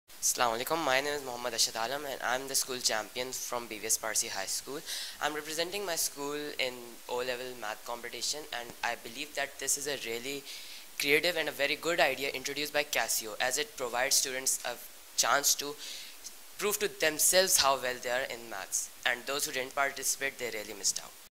Muhammad Ashhad Alam
Muhammad Ashhad Alam, School Champion from BVS Parsi High School, shares his views about the Learnaration Maths & Accounting Competition 2017 held at Dawood Public School A-Level on March 4,2017